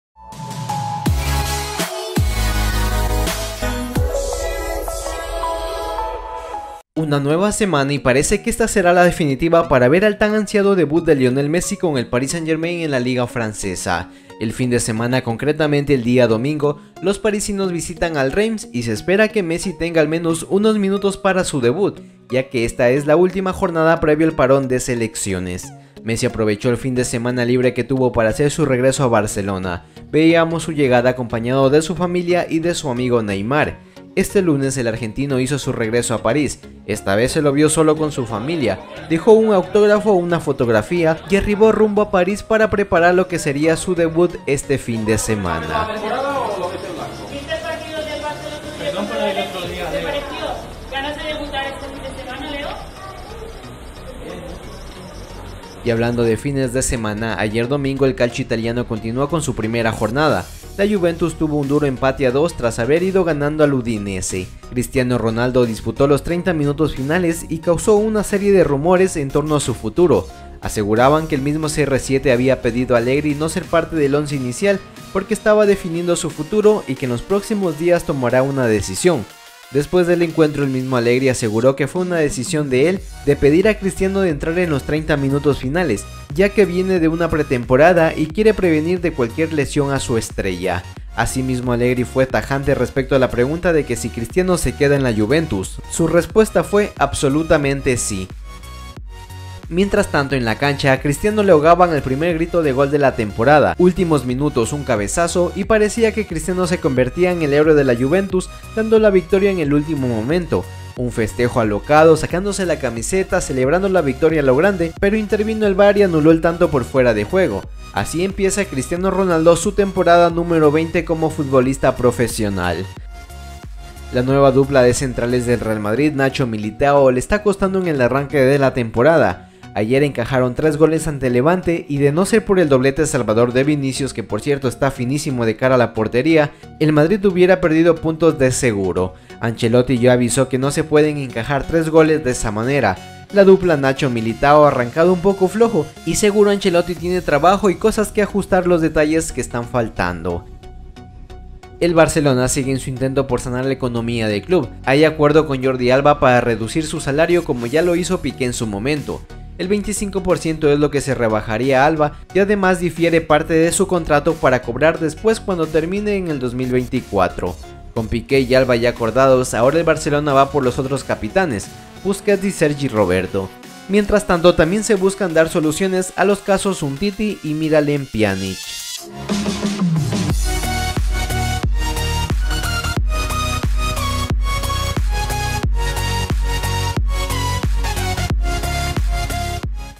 MESSI REGRESA a PARÍS a PREPARAR su DEBUT | CRISTIANO “SE QUEDA” en la JUVE | ALBA se BAJA SALARIO
Leo Messi ha emprendido este lunes el viaje de regreso a París, tras disfrutar del fin de semana en Barcelona y Castelldefels. El argentino ha llegado al aeropuerto del Prat para tomar un vuelo con dirección a la capital gala, donde retomará los entrenamientos a las órdenes de Mauricio Pochettino.

Si no hay contratiempo de última hora, el crack argentino debutará con la camiseta el PSG este próximo domingo, en el partido correspondiente a la cuarta jornada de la Ligue 1 que enfrentará a los parisinos con el Reims en feudo de este último equipo.

Tres jornadas en la Ligue 1 y el mundo del fútbol, sobre todo el francés, sigue a la espera de ver a Leo Messi con su nueva camiseta sobre un terreno de juego. Aún es pronto, según los técnicos del club parisino, sin embargo, y a tenor de las últimas declaraciones de Mauricio Pochettino parece que queda poco para que llegue por fin ese momento.

"Los nuevos se fueron acoplando al equipo y hay que manejar esos niveles. Más allá de eso estoy muy contento de haber ganado y tener nueve puntos en tres partidos", dijo para los micrófonos de ESPN.

Y es que el comienzo del PSG en la competición doméstica, en la que el mismo técnico argentino asume que son el rival a batir, no ha podido ser mejor. Tres victorias de tres partidos. El último, ante el Brest por un total de 2-4, en el que el equipo que dirige Pochettino volvió a encajar, eso sí, dos tantos, y repitió el mismo resultado que ante el Estrasburgo.

Suma y sigue
Así las cosas, el equipo sigue sumando y el hecho de que se incorpore Messi al equipo supone que, si ya el equipo en Francia es muy superior, con el astro argentino todo se multiplica por mil. Y el caso es que todo apunta a que el Reims sea su primer rival con la camiseta del PSG.

"La semana ha sido muy buena para Leo, por lo que esperamos que la semana que viene es larga y esperamos que si va todo bien pueda estar en el grupo y empiece a ser parte del equipo en la parte competitiva".